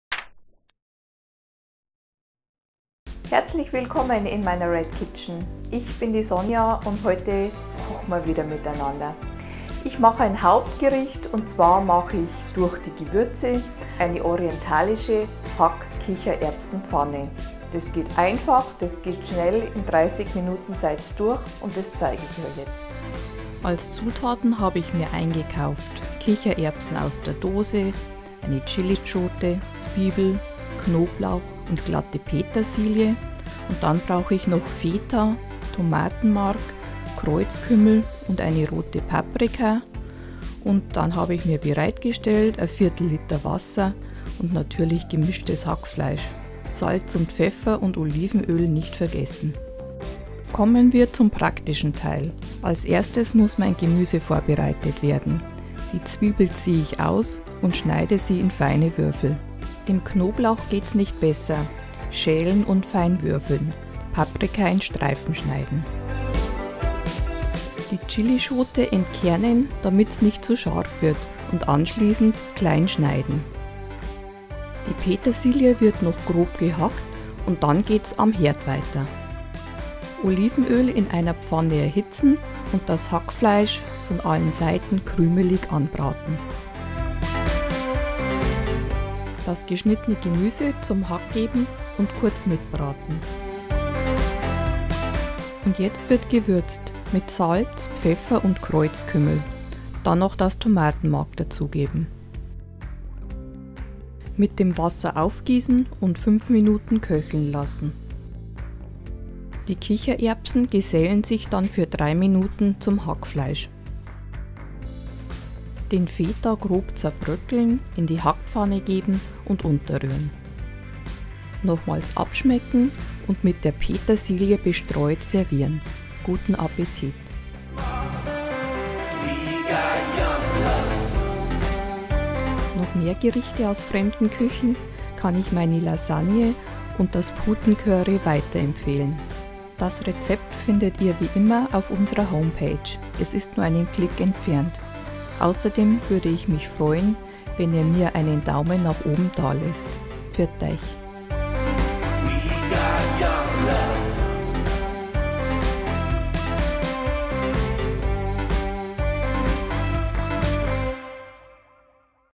Rezept - Orientalische Hack-Kichererbsen-Pfanne (Red Kitchen - Folge 282)
Schnell und einfach ist diese Hackpfanne zubereitet.

Zutaten für 4 Personen: 1 Zwiebel, 1 Knoblauchzehe, 1 große rote Paprika, 1 grüne Chilischote, 2 El Olivenöl, 600 g Hackfleisch, Salz, Pfeffer, 1-2 Tl gemahlener Kreuzkümmel, 2 El Tomatenmark, 1 Dose (425 ml) Kichererbsen, 1/2 Bund glatte Petersilie, 200 g Feta

Unbedingt vorbeischauen!